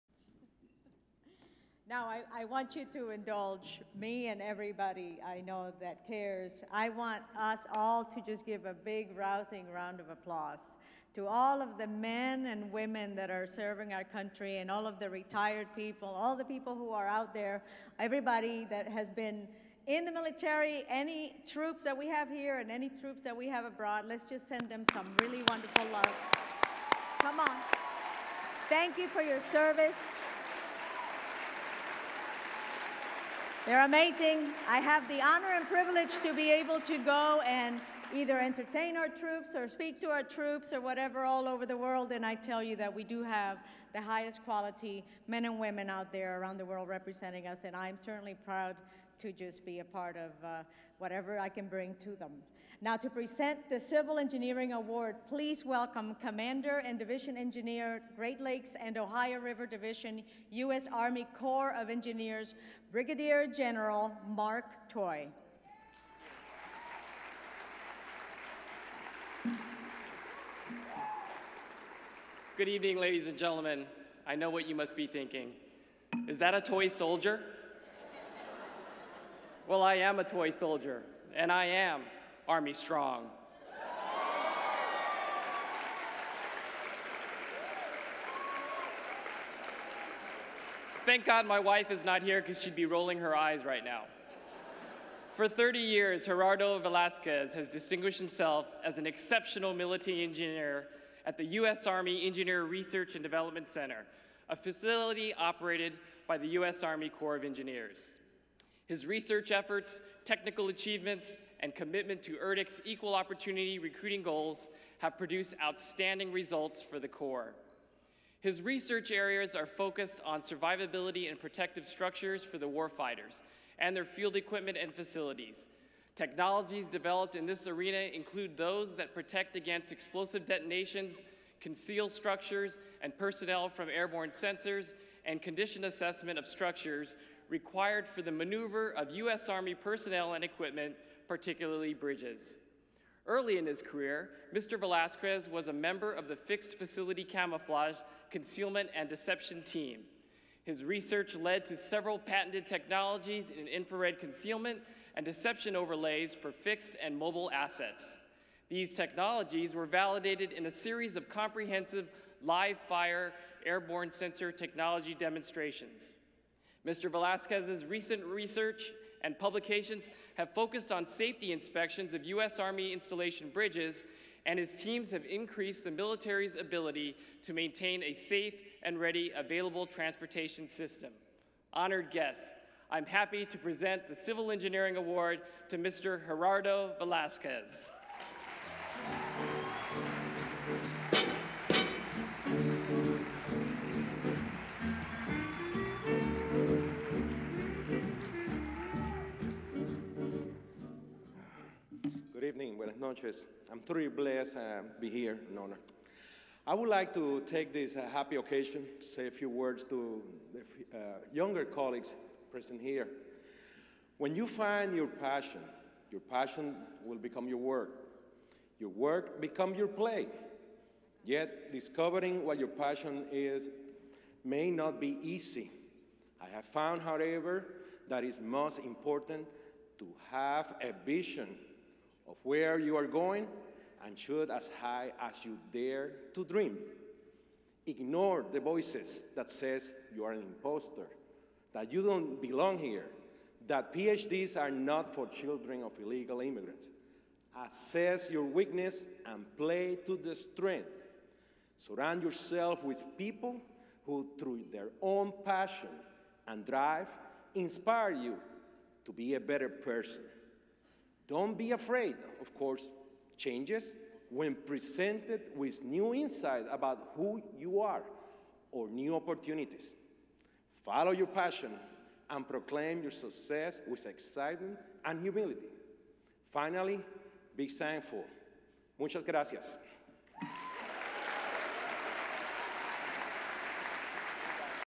2016 HENAAC Awards Show 8/35: Civil Engineering Gerardo Velazquez, USACE
Brigadier General R. Mark Toy, Commander & Division Engineer, Great Lakes and Ohio River Division, U.S. Army Corps of Engineers, presents the Civil Engineering Award to Gerardo I. Velázquez, Research Civil Engineer, Structural Engineering Branch, Geosciences and Structural
Division, Geotechnical and Structures Laboratory, U.S. Army Engineer Research & Development Center, U.S Army Corps of Engineers.